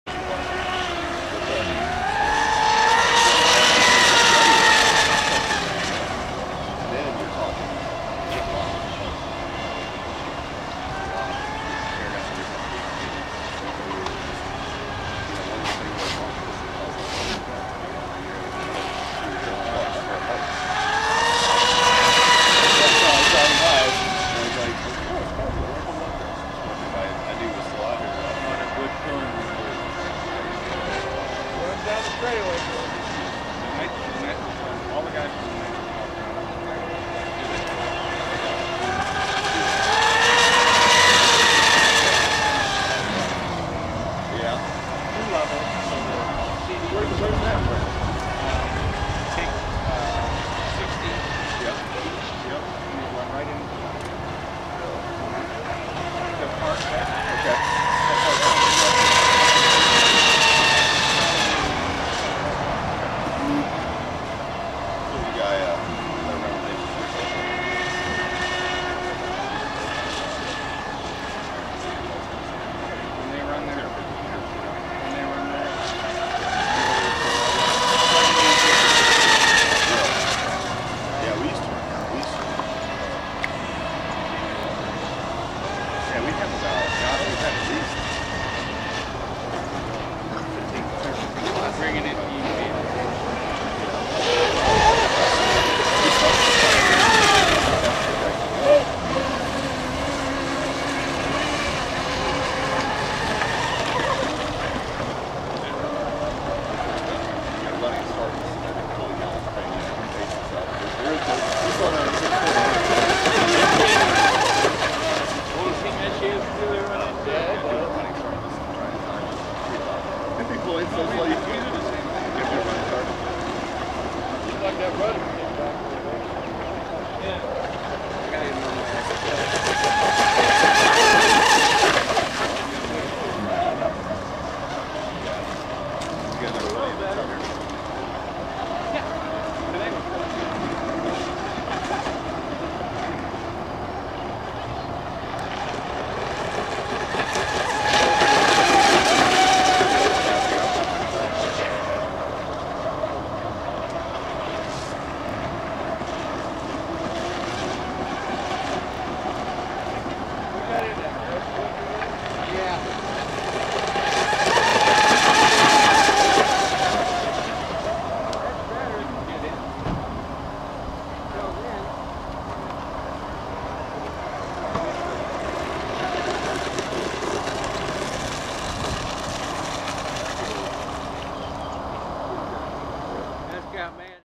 EKOS Twin and Popeyes Hydro
Started off shooting Richard's EKOS twin and about the time he was coming in Chris went out again with his Popeye's Hydro